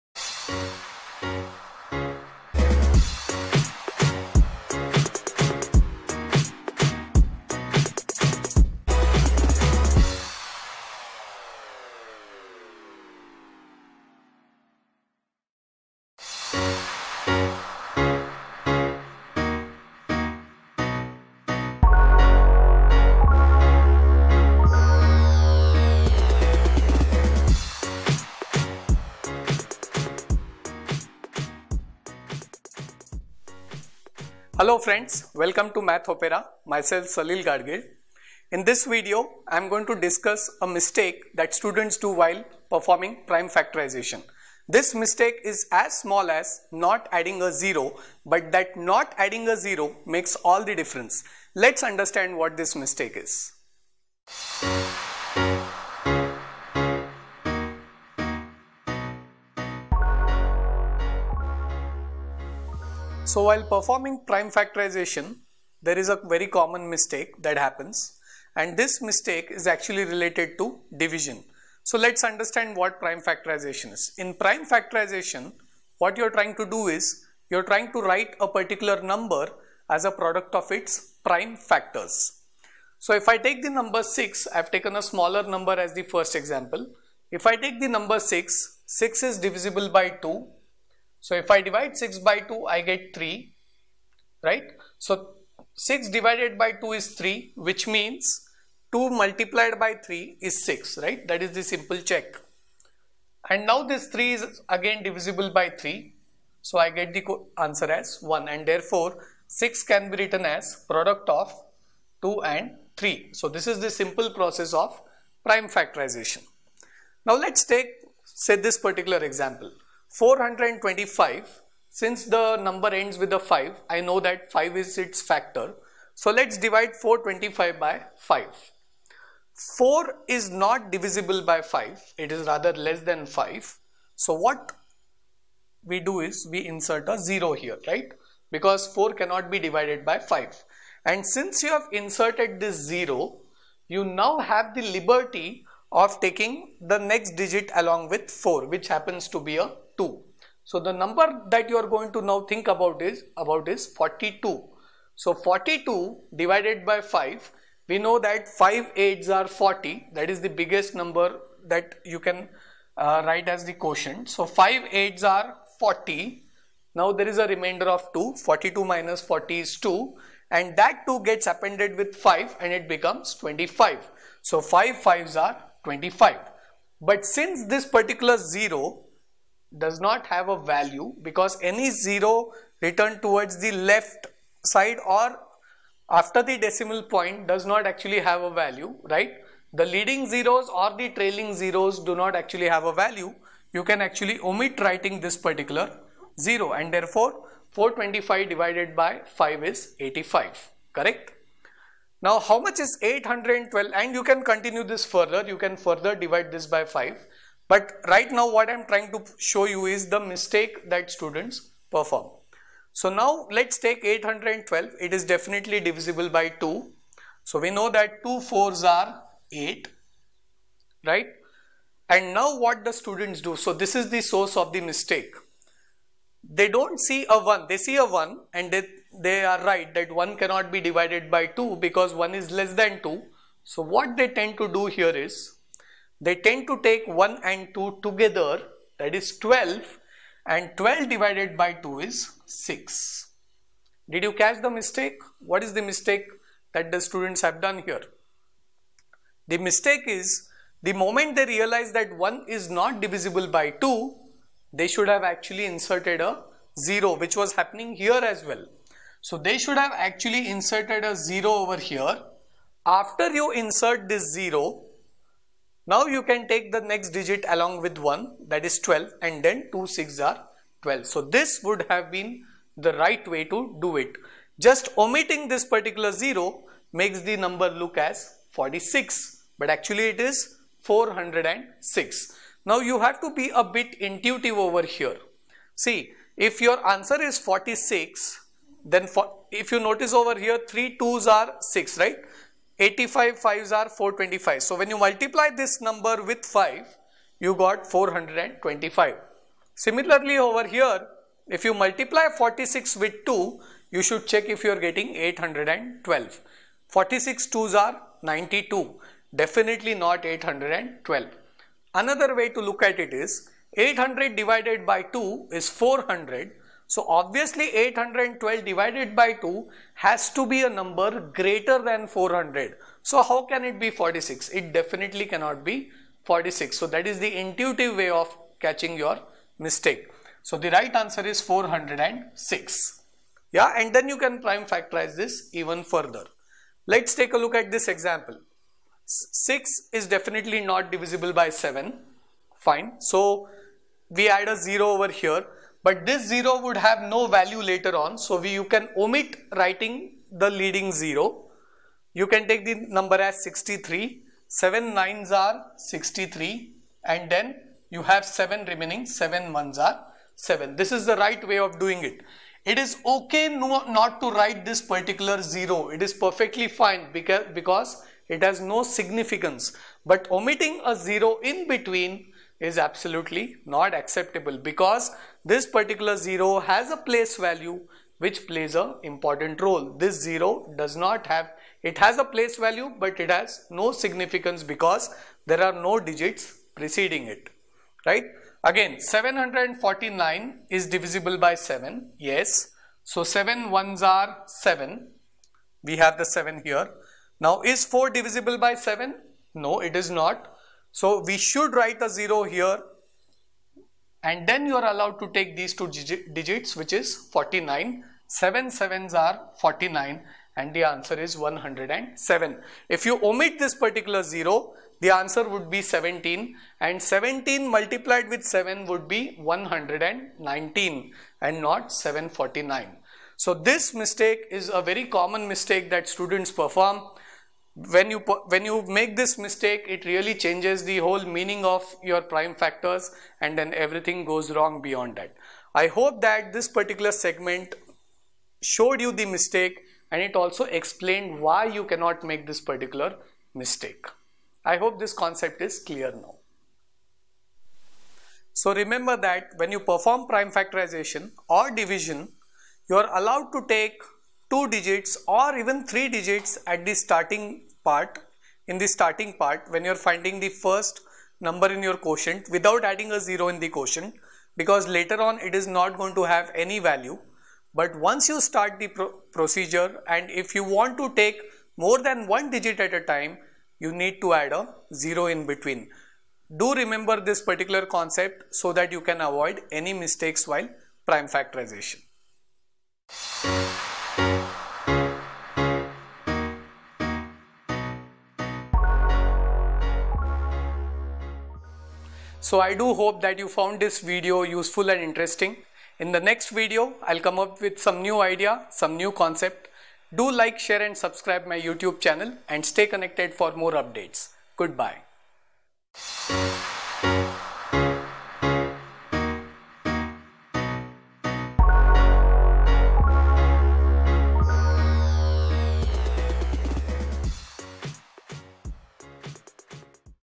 Math Opera Episode 17: Mistake in Prime Factorisation
When students prime factorize a number, there is a very common mistake they commit at times - which could be not adding a zero. This mistake can be fatal as it disturbs the complete factor tree. In this video I explain the mistake and how this mistake could be caught in a very intuitive manner.

I hope this clears up the air and that students would avoid making this mistake in the future. Enjoy the video in HD for better viewing.